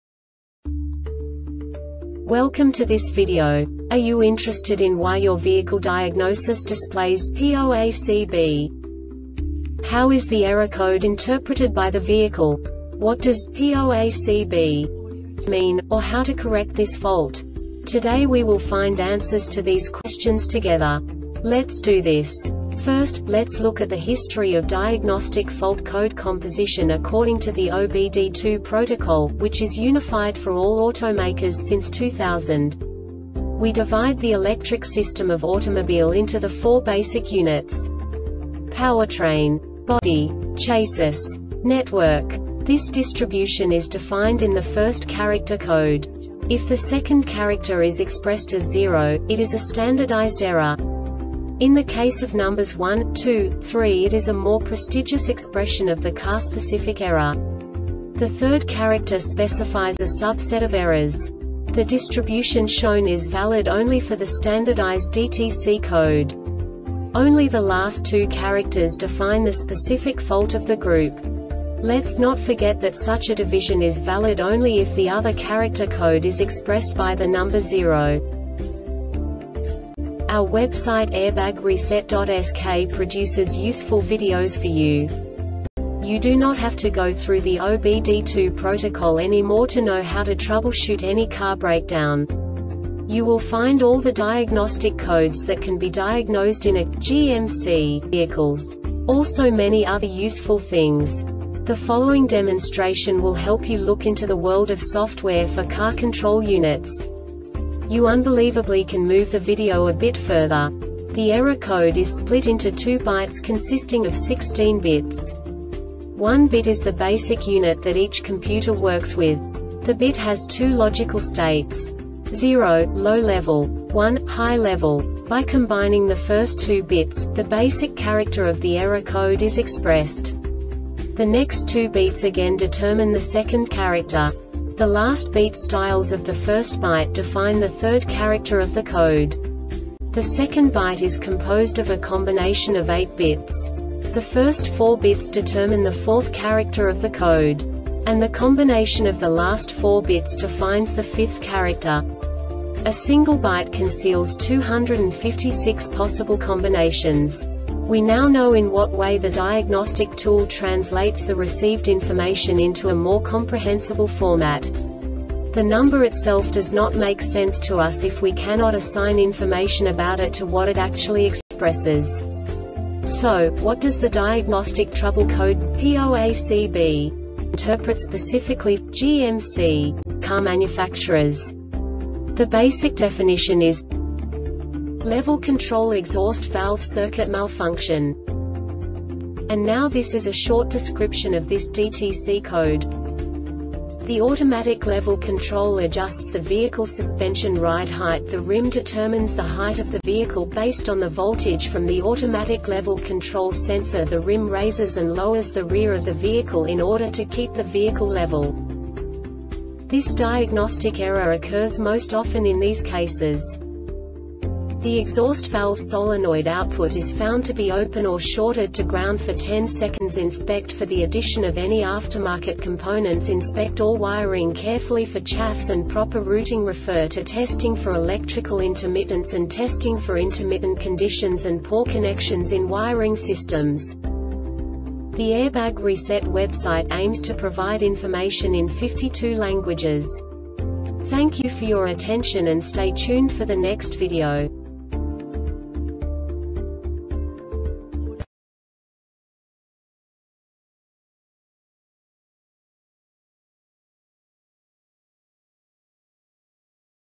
DTC GMC P0ACB Short Explanation
Contents:
0:21 Basic DTC analysis according to OBD2 protocol standard.
1:48 Insight into programming
2:58 A diagnostically understandable description of the Diagnostic Trouble Code
3:05 Basic error definition

Make:
GMC
Code:
P0ACB
Definition:
Level Control Exhaust Valve Circuit Malfunction
Description:
The automatic level control adjusts the vehicle suspension ride height. The RIM determines the height of the vehicle based on the voltage from the automatic level control sensor. The RIM raises and lowers the rear of the vehicle in order to keep the vehicle level.
Cause:
The exhaust valve solenoid output is found to be open or shorted to ground for 10 seconds.
Diagnosis:
Inspect for the addition of any aftermarket componentsInspect all wiring carefully for chaffs and proper routing. Refer to Testing for Electrical Intermittents and Testing for Intermittent Conditions and Poor Connections in Wiring Systems